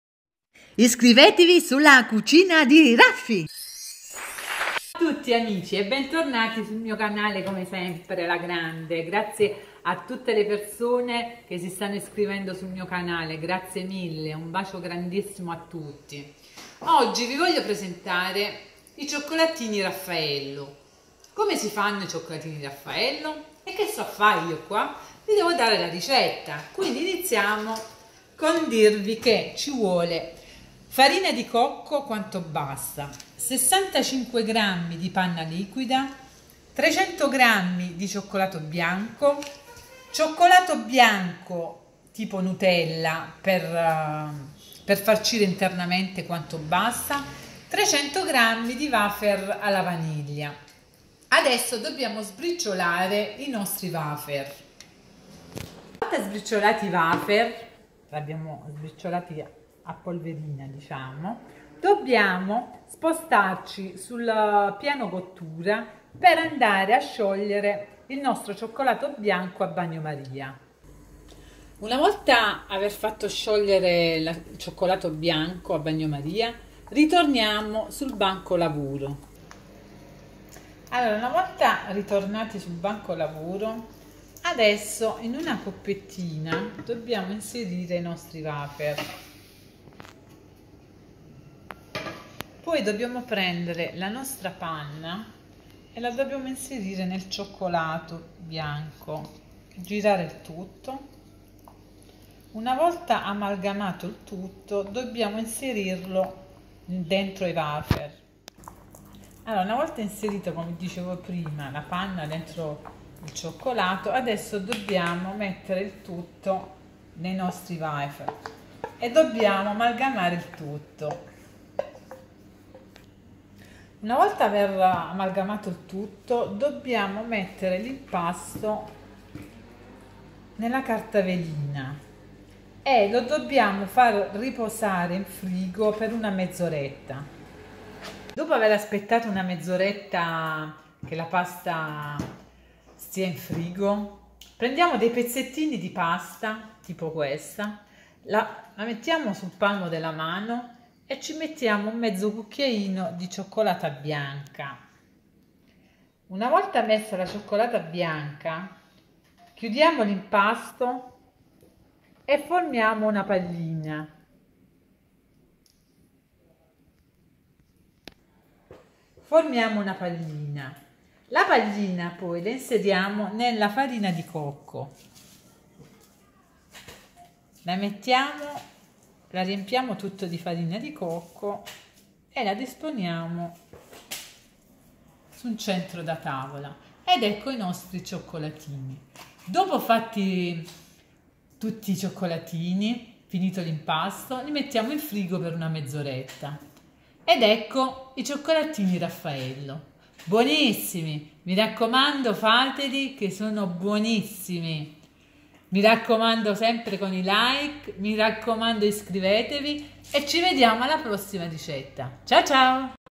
Cioccolatini Raffaello
INGREDIENTI
-farina di cocco quanto basta
-65 ml di panna liquida
-300 g di cioccolato bianco
-nutella bianca
-300 g di wafer alla vaniglia